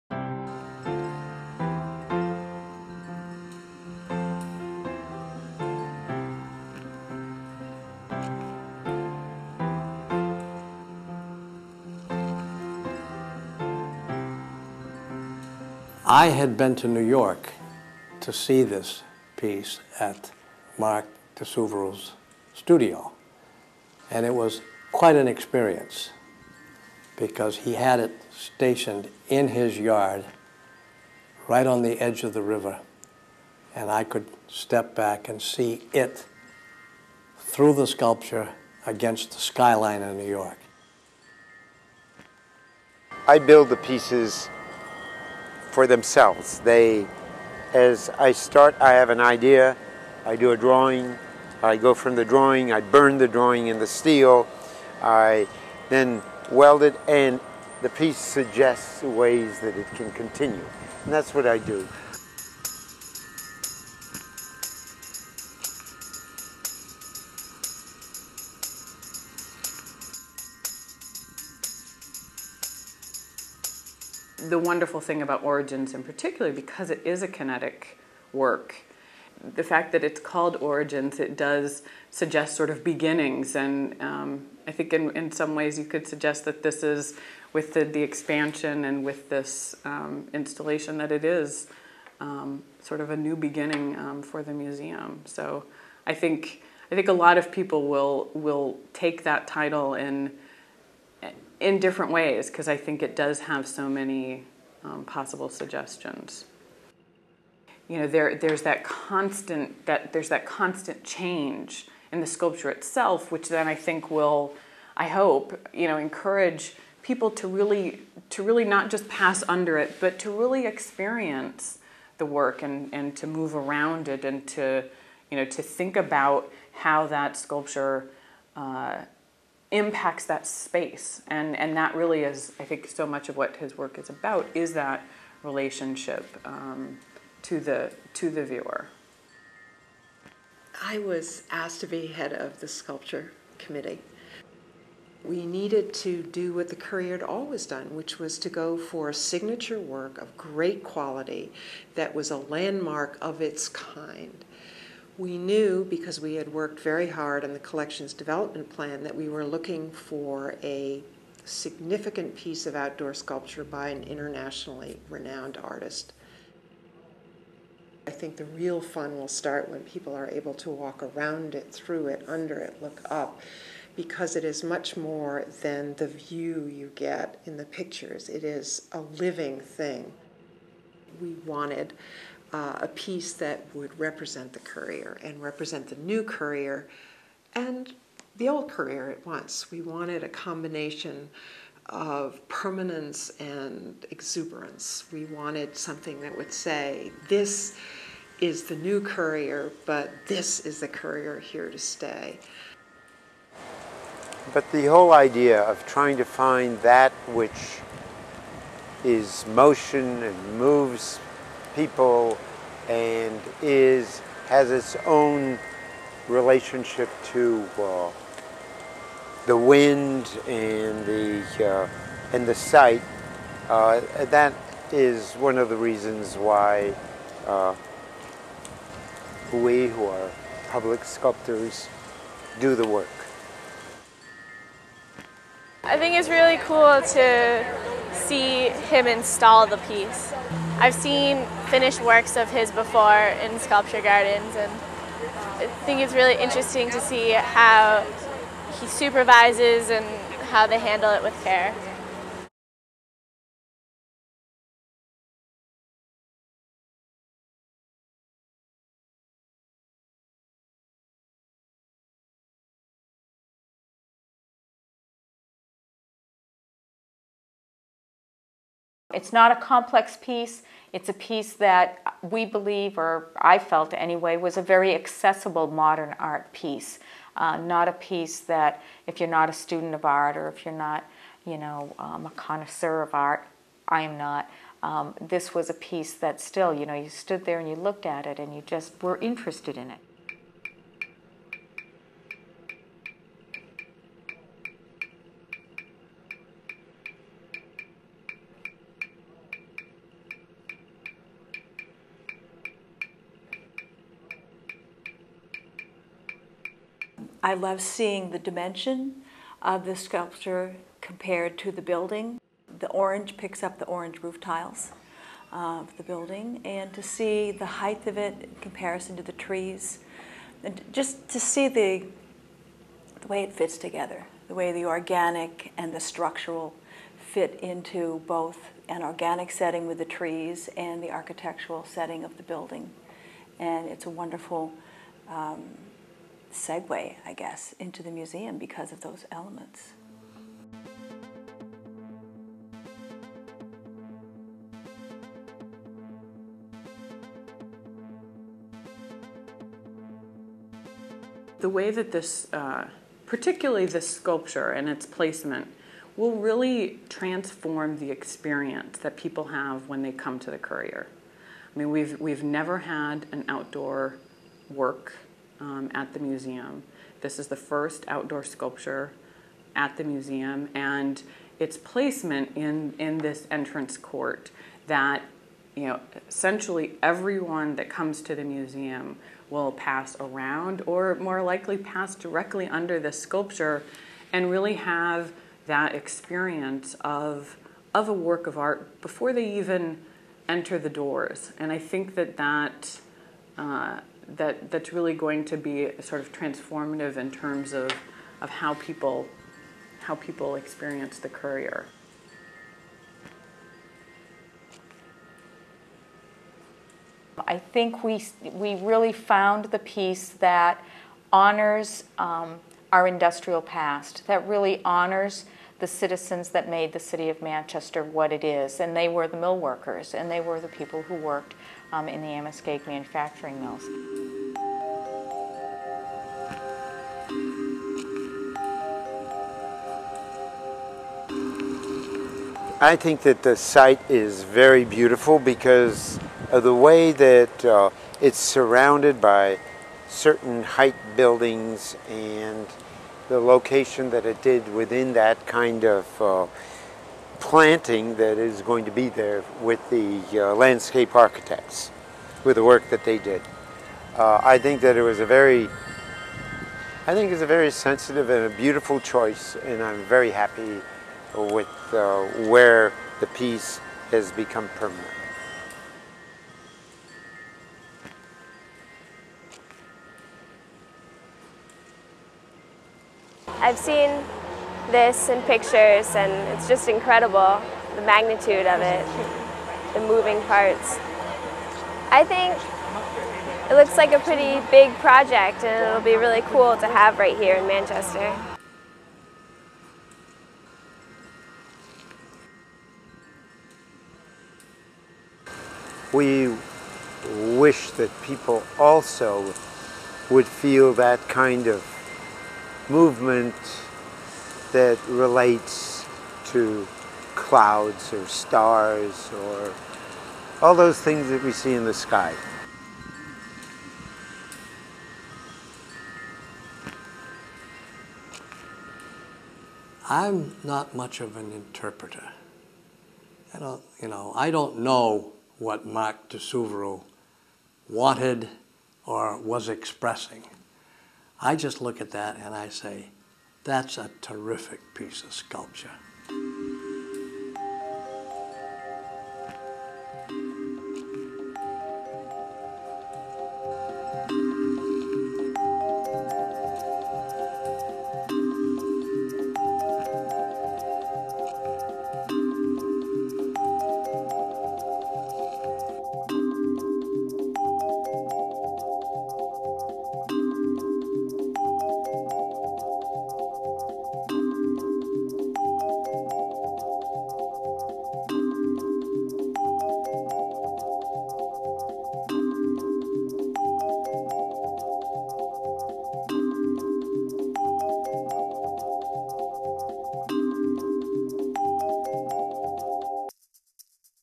Currier Museum DiSuvero installation
A short film on the installation of a sculpture of Mark diSuvero at the Currier Museum in New Hampshire.